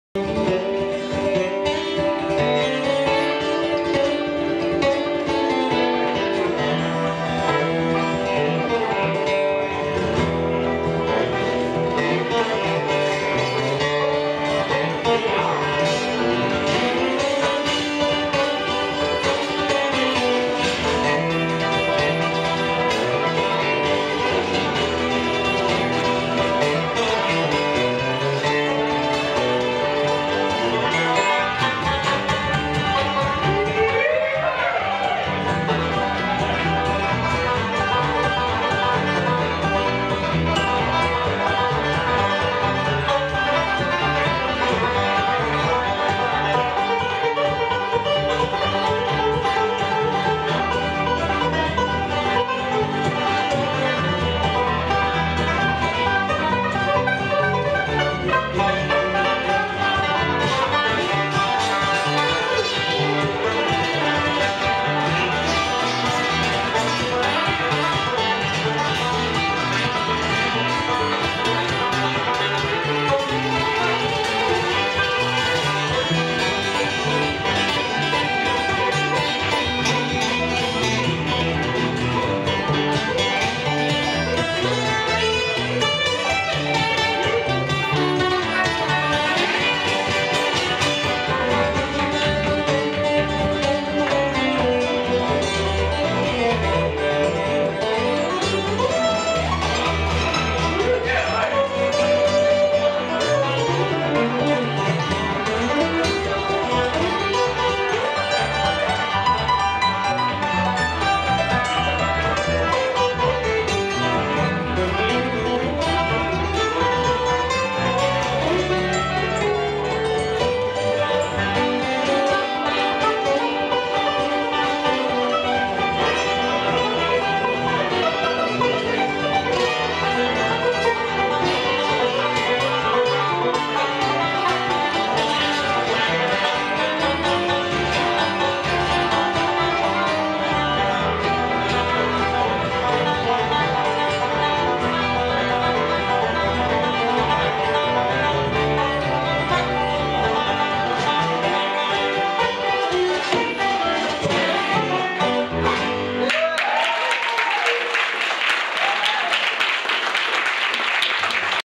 Well Strung (Gavin, Mazengarb, Speir) at the WBS
Well Strung (Nigel Gavin, Mark Mazengarb, Colin Speir) performing at the Wellington Bluegrass Society, Petone New Zealand, 30 November 2013.